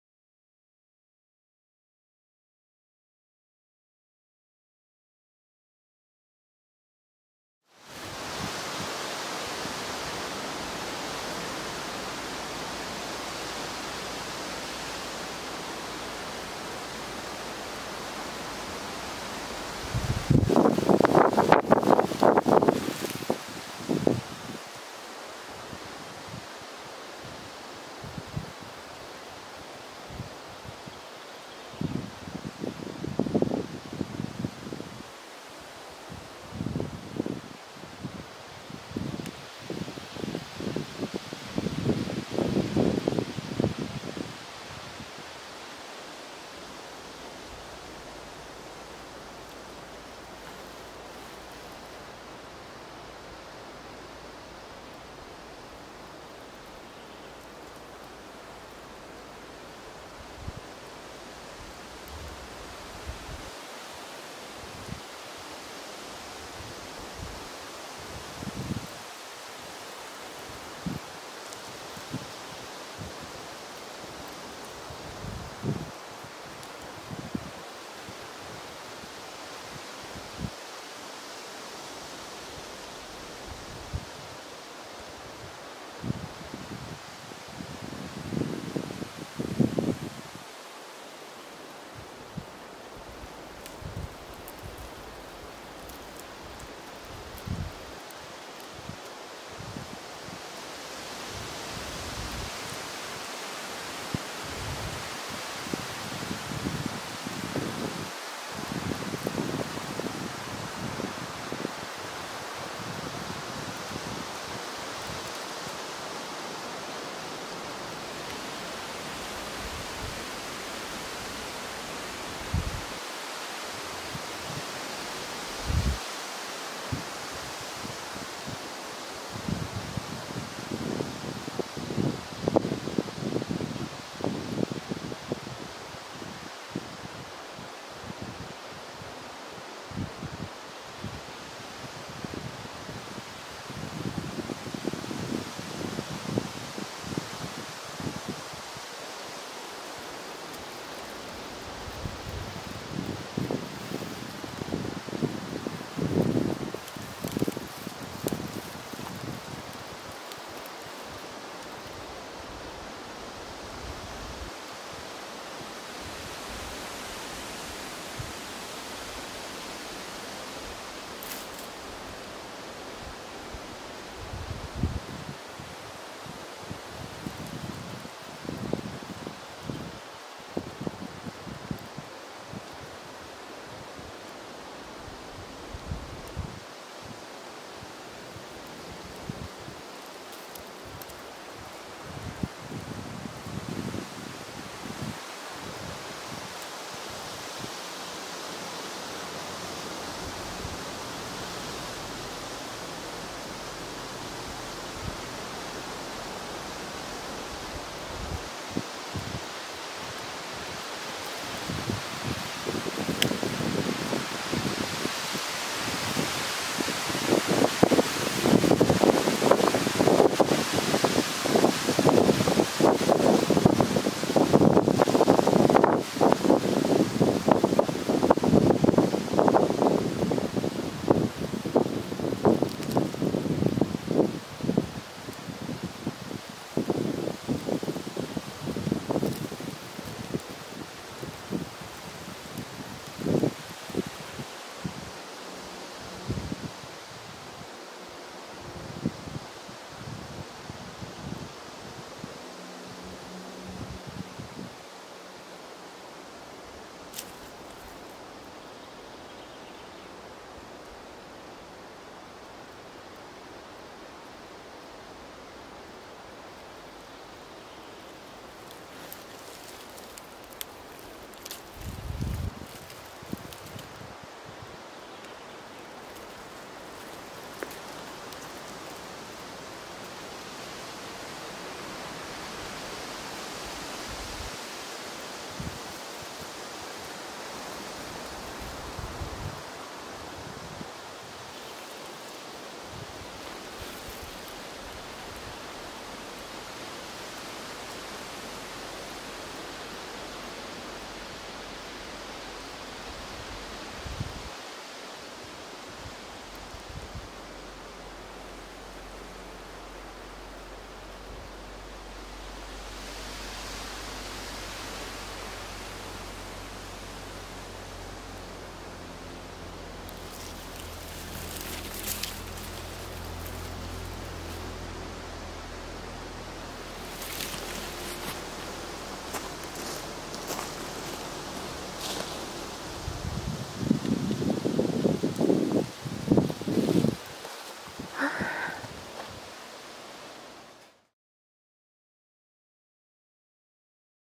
Hongatar project/Work in progress 2
At Nuuksio's forest continuing with this my artistically research in collaboration with the costume designer Joanna Weckman who work with recycled materials and traditional techniques.
Hongatar project gives space to body thinking, body awareness and cellular memories to be connected with and expressed.